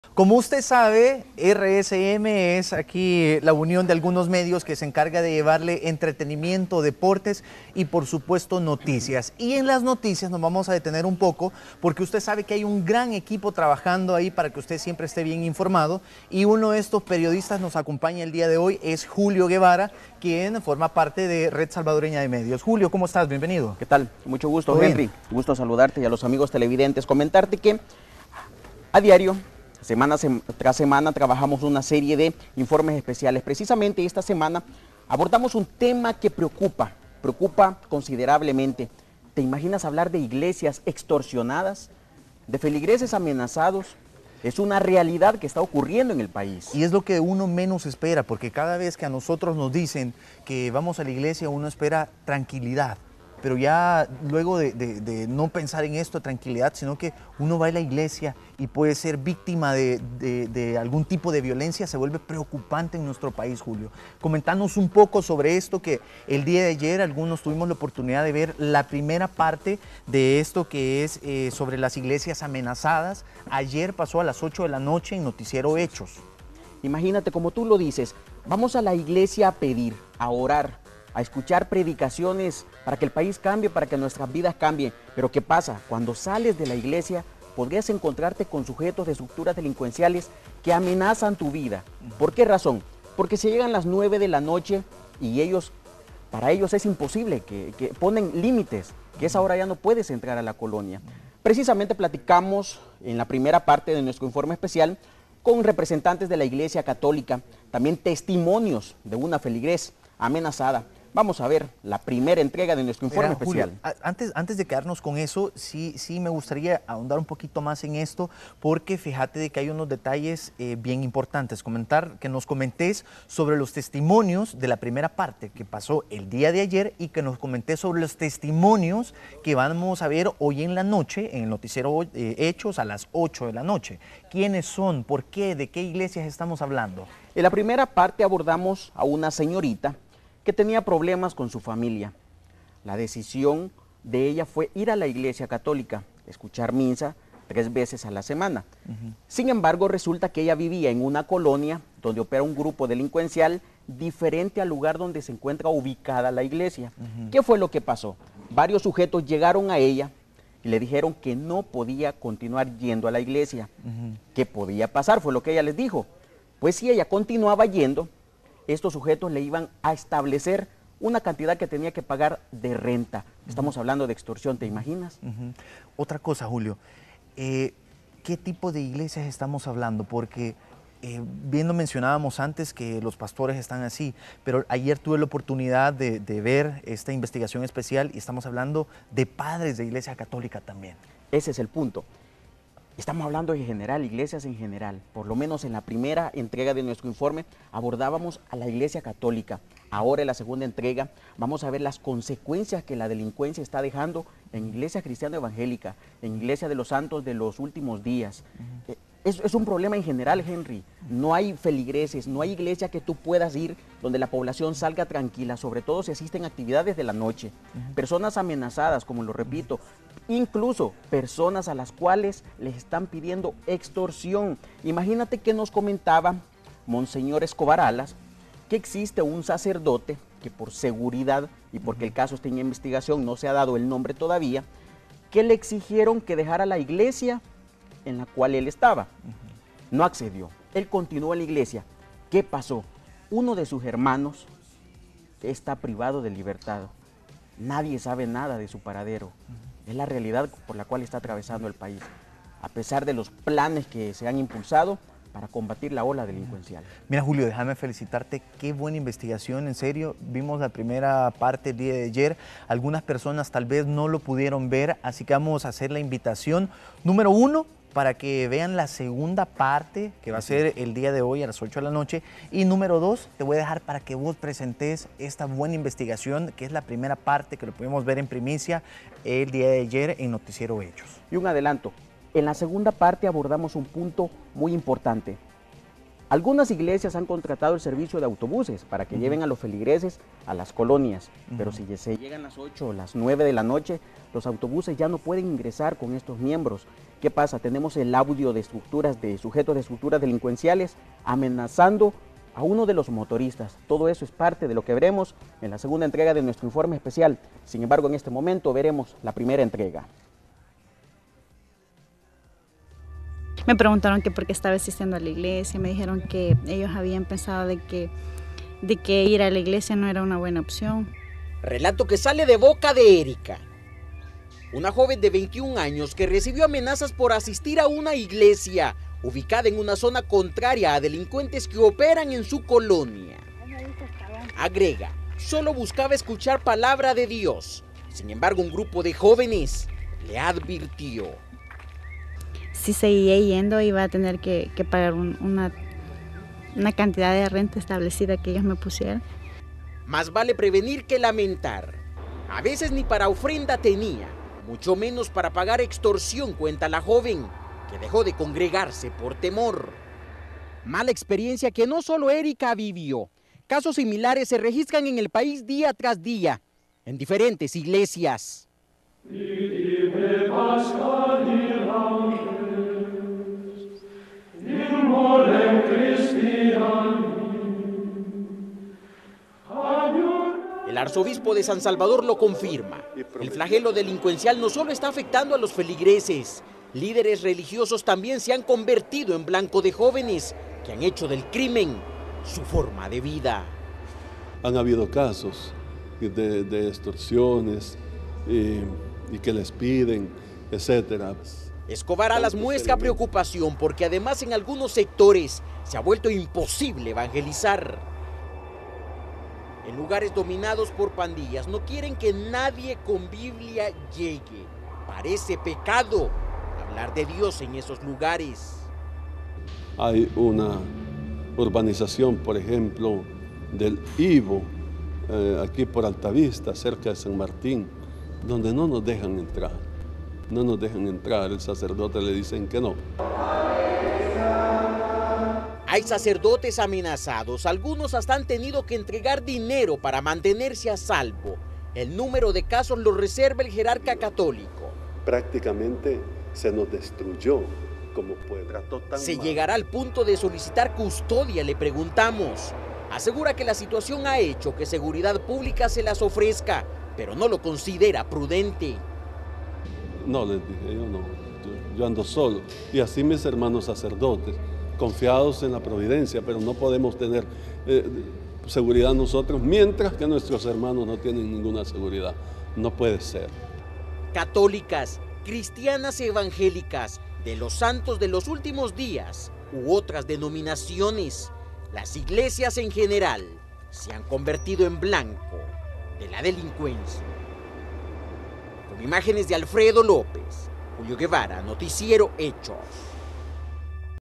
Iglesias amenazadas por la delincuencia en el país
Hoy nos acompaña el periodista, Julio Guevara, perteneciente a RSM. En esta oportunidad la visita es por un tema preocupante para la sociedad salvadoreña. Esta es una realidad que está ocurriendo en el país.